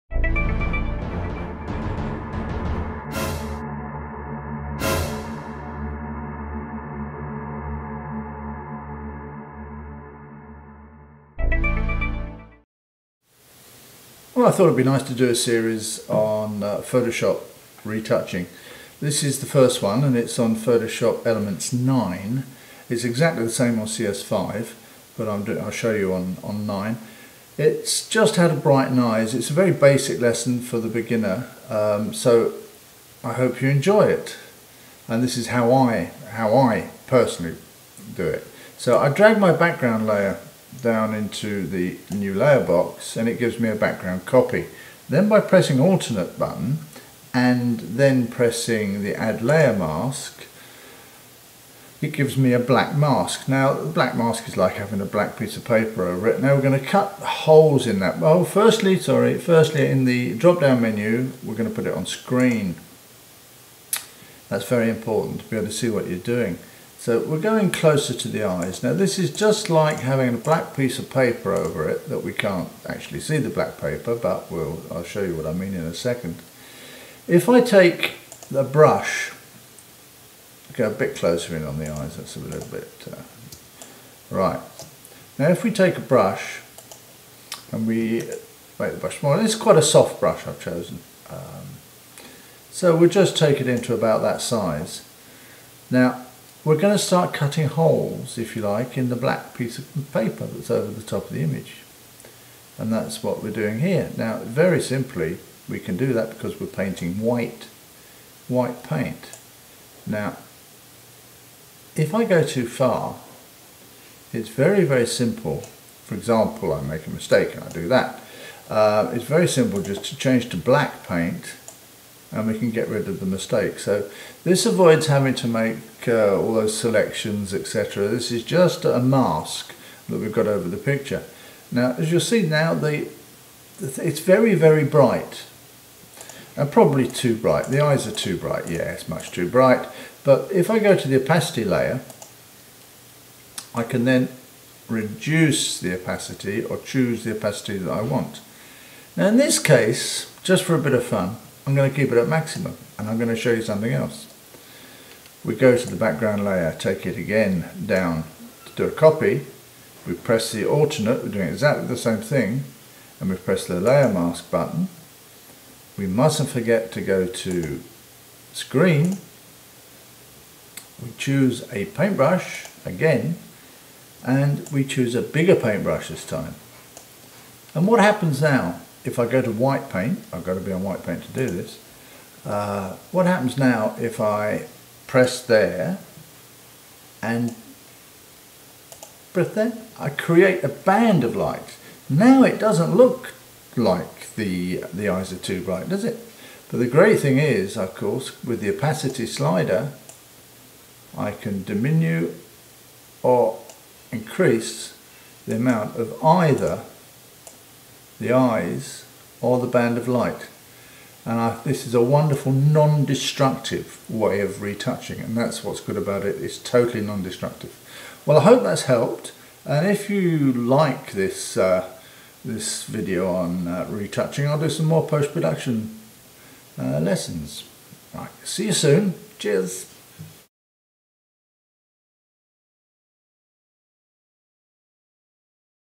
Retouching eyes with Photoshop using layer masks
A  lesson on how to use a layer mask to brighten eyes in Photoshop ,this tutorial shows that by using a layer mask we can use a non destructive method of retouching eyes on a portrait photo,any studio or exterior image can be retouched using this simple method of retouching on layers.